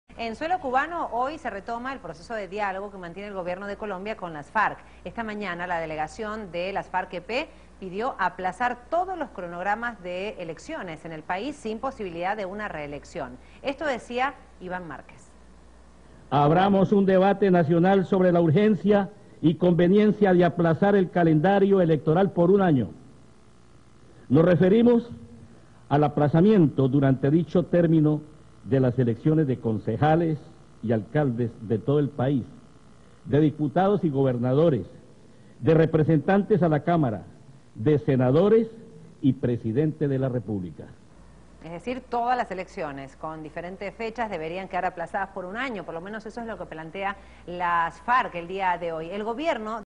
FARC propõem prorrogar eleições na Colômbia por um ano
As Forças Armadas Revolucionárias da Colômbia, FARC, pediram que as eleições municipais, estaduais e presidenciais fossem prorrogadas por um ano em todo o país para que não interferissem no processo de paz que se desenvolve em Cuba. teleSUR